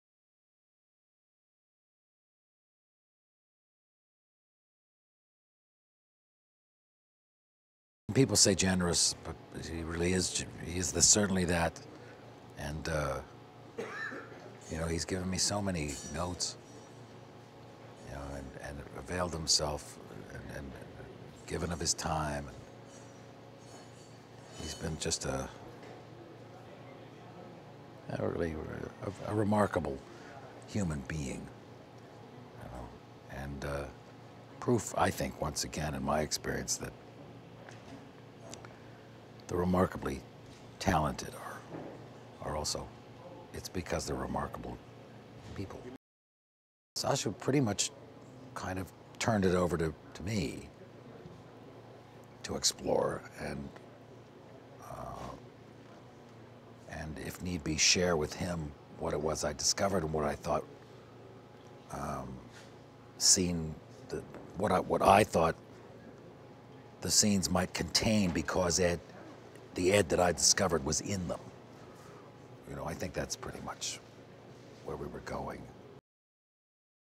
Michael Wincott's Official "Hitchcock" Interview - Celebs.com
The new "Hitchcock" film stars Anthony Hopkins as the acclaimed auteur.

for more interviews, images and news of your favorite personalities.